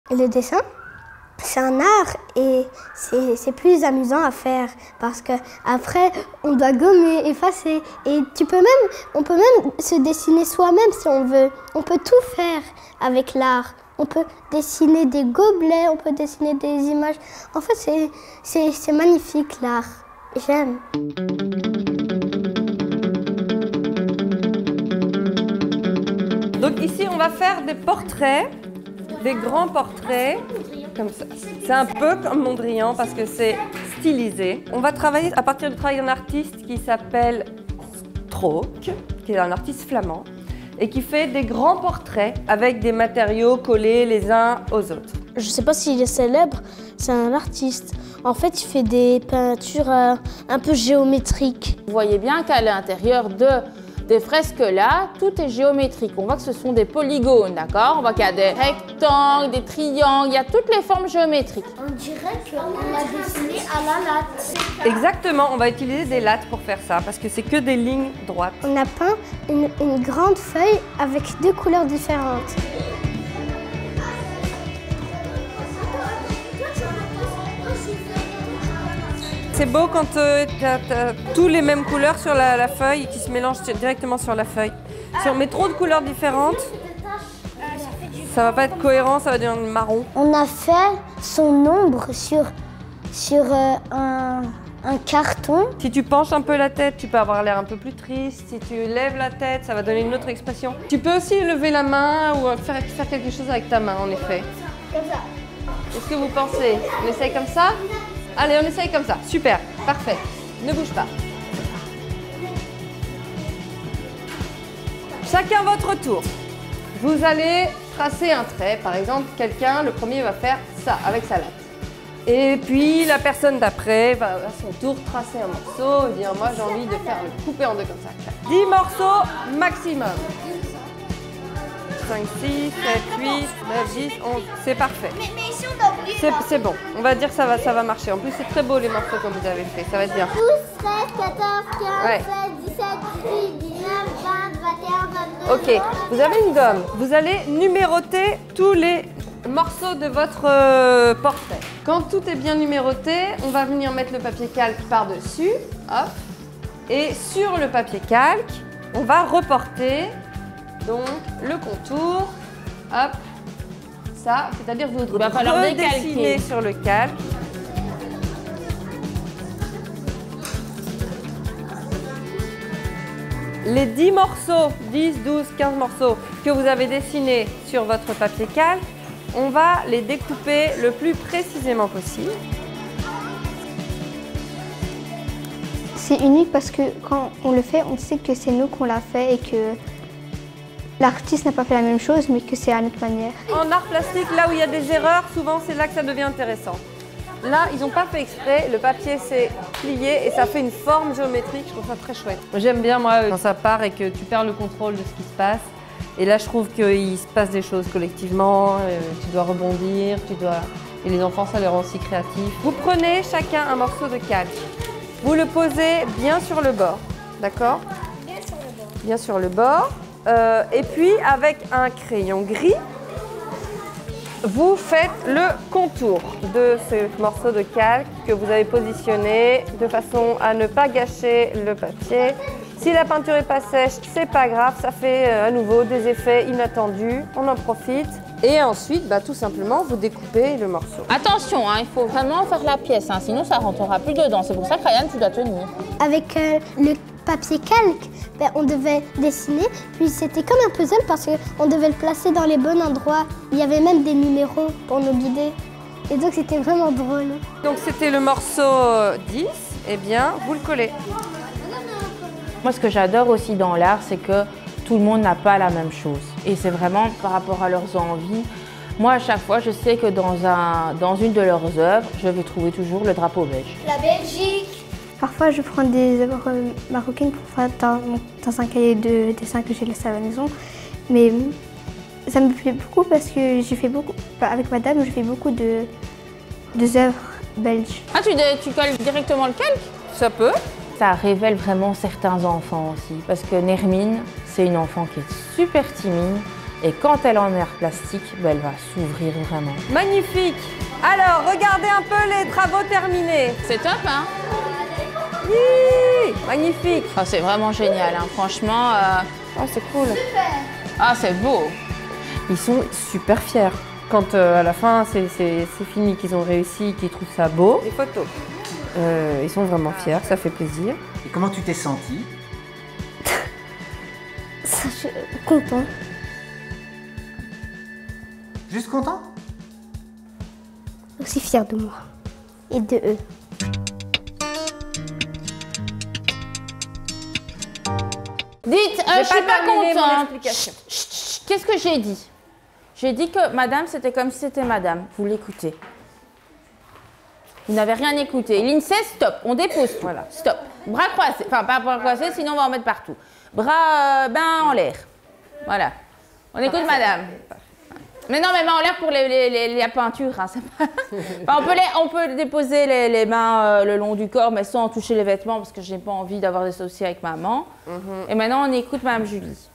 À la manière de Strook - Arts plastiques en primaire
Dans le cadre de Mon Patrimoine Revisité, la scénographe Julie Michaud a proposé aux enfants de l'école communale Armand Swartenbroeks (Koekelberg) un atelier arts plastiques s'inspirant du travail de l'artiste belge Stefaan De Croock alias Strook.
Avec la collaboration de Archipel 19 (Centre culturel de Berchem-Sainte-Agathe et Koekelberg).